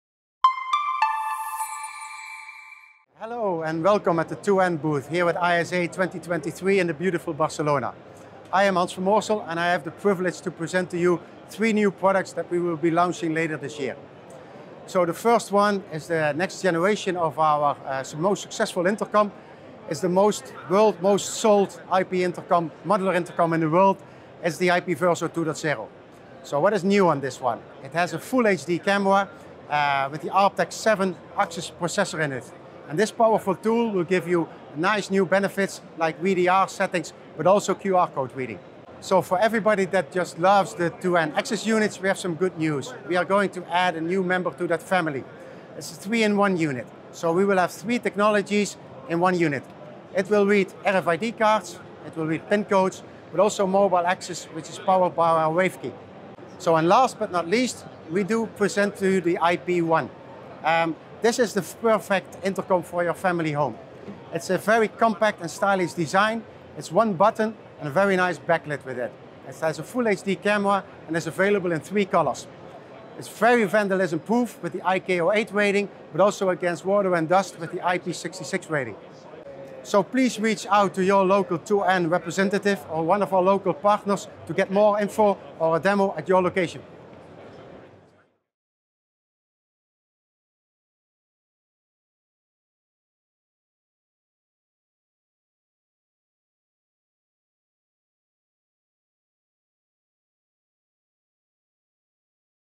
ISE 2023 | 2N Product Showcase | Good things come in threes
Hans van Moorsel details three additions to the 2N range of solutions comprising an IP modular intercom with a full HD camera and a powerful processor, a 3-in-1 unit that handles RFID, pin codes and mobile access, and a rugged intercom system for the home.